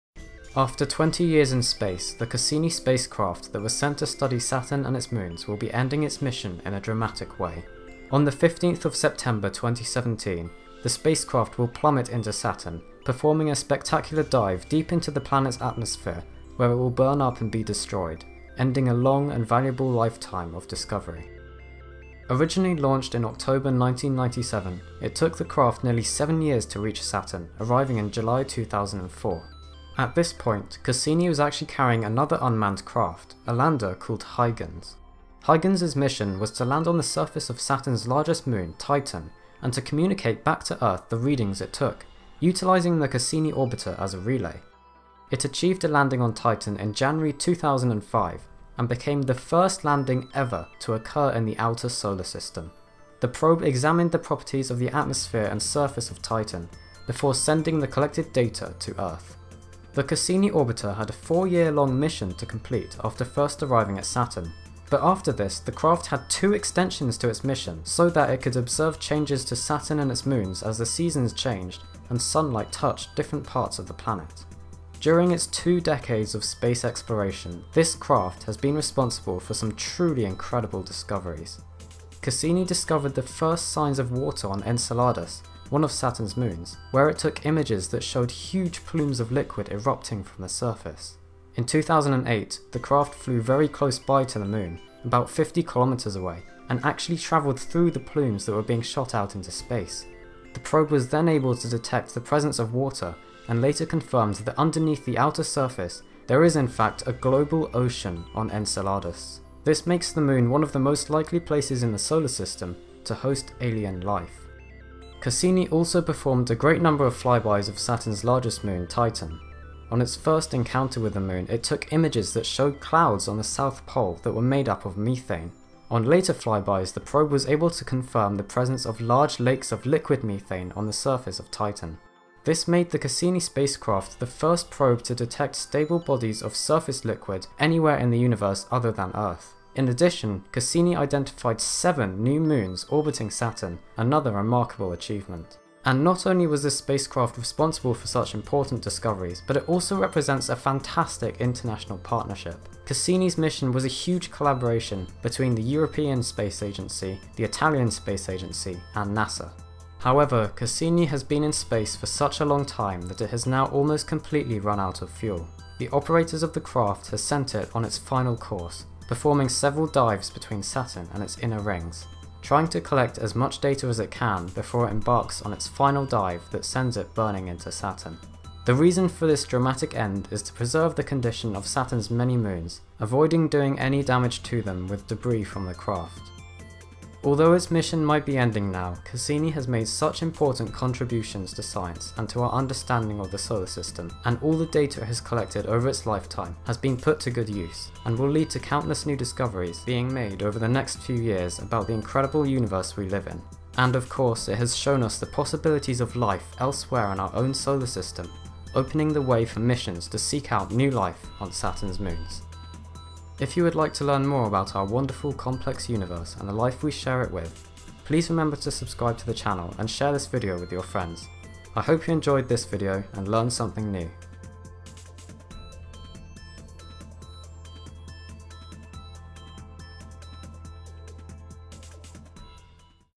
Cassini - The Probe That Crashed Into Saturn
The Cassini spacecraft has been on a mission lasting 20 years and has made many important discoveries, including the potential for alien life on Enceladus. But now, almost out of fuel and stranded in deep space, it is being sent on a crash-course into Saturn.